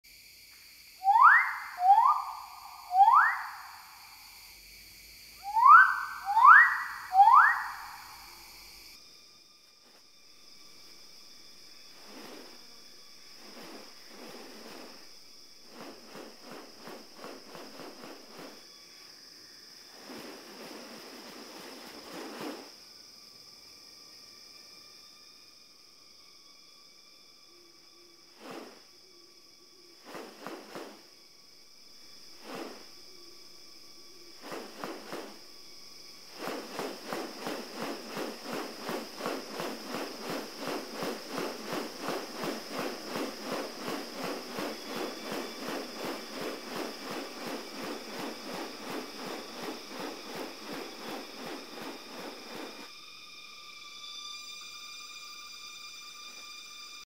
Beautiful Magnificent Riflebird Mating Dance
Beautiful Magnificent Riflebird Dance to Impress Their Mates

The magnificent riflebird is a species of passerine bird of the family Paradisaeidae. Magnificent riflebirds are widely distributed throughout lowland rainforests of western New Guinea and the northern Cape York Peninsula. Wikipedia

Scientific name: Ptiloris magnificus
Conservation status: Least Concern (Population stable) Encyclopedia of Life
Higher classification: Ptiloris
Family: Paradisaeidae
Order: Passeriformes